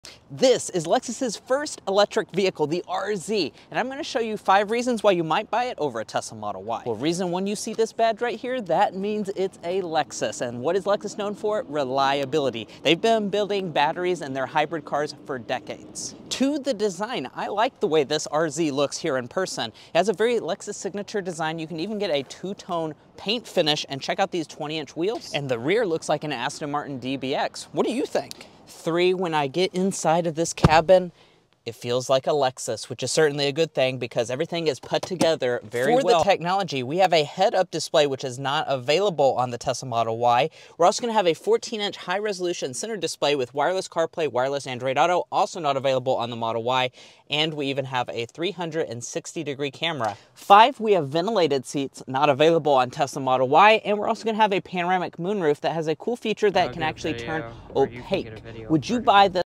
Lexus' first EV is better than a Tesla in some ways... but not in every way...
Check out our Full Review of the 2023 Lexus RZ 450e! This is Lexus' first EV and is it better than a Tesla??